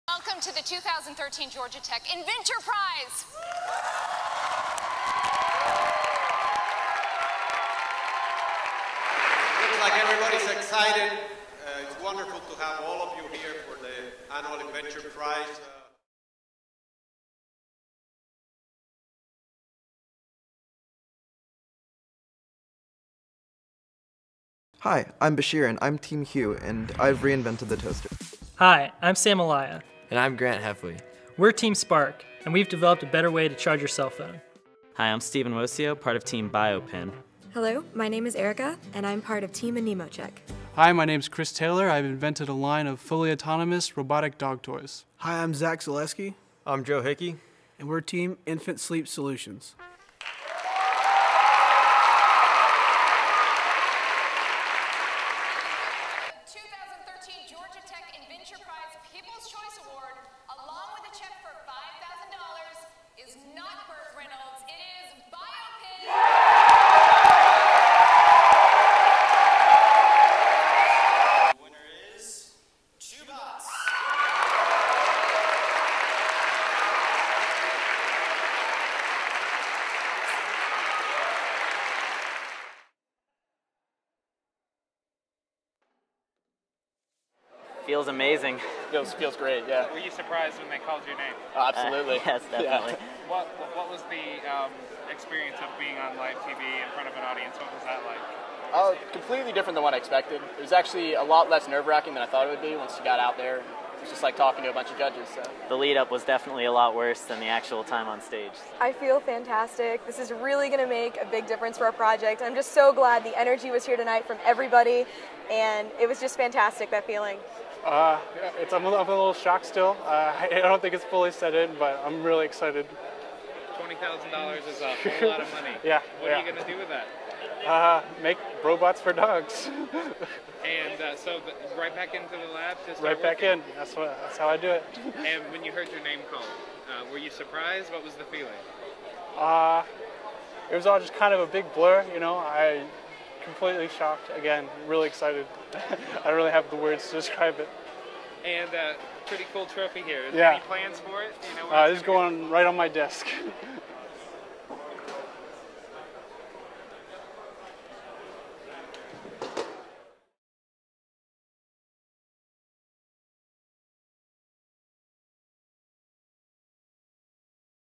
2013 InVenture Prize at Georgia Tech Finals
6 finalists competed to see which team's invention would come out on top.